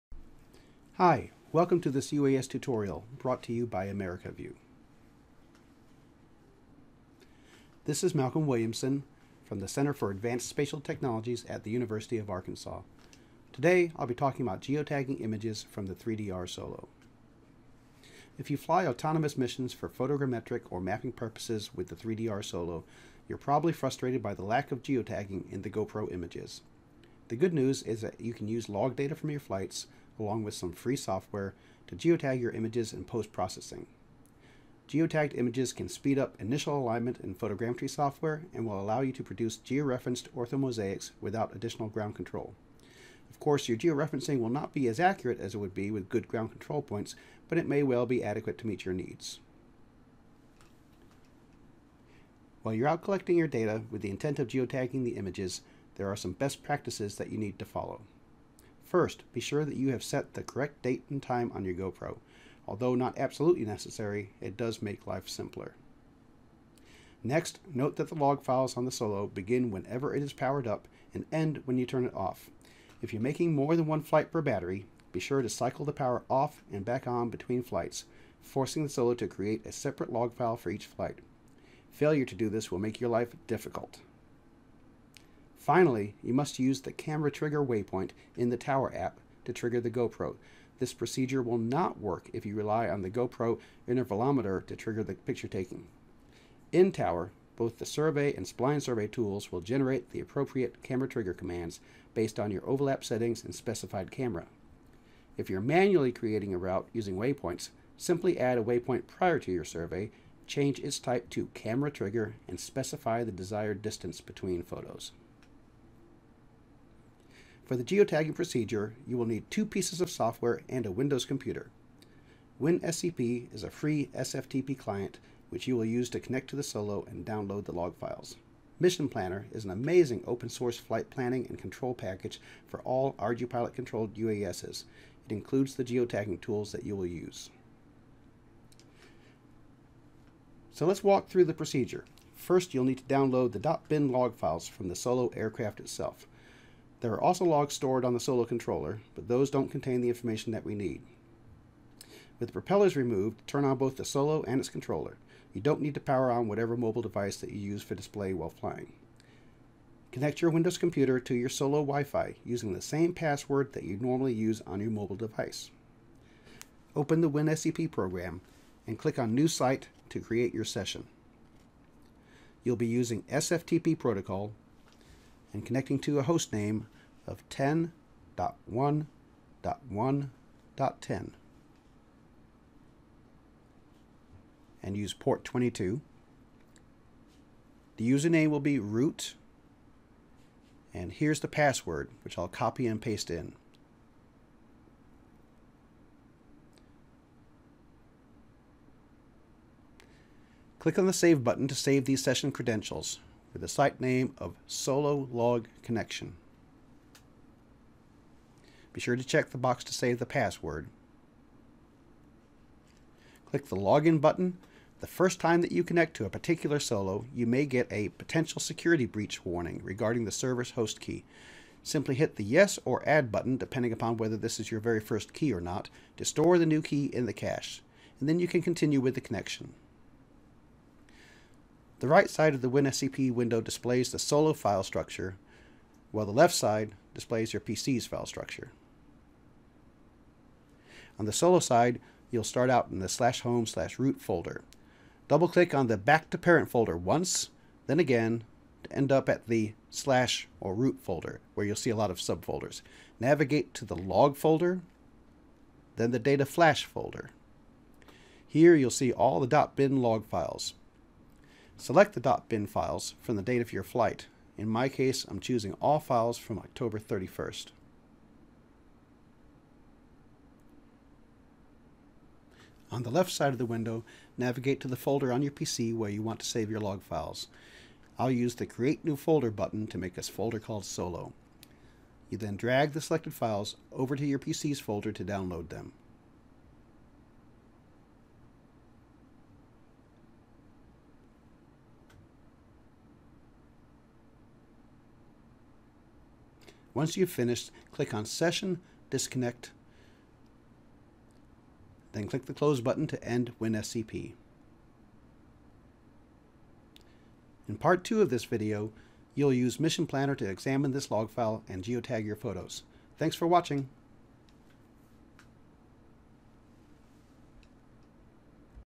Geotagging Images from the 3DR Solo: Part 1
Tutorial video funded by AmericaView, Inc. The video, produced through the ArkansasView program was developed by Malcolm Williamson from the Center for Advanced Spatial Technologies at the University of Arkansas. The project Principal Investigator is Dr. Jason A. Tullis.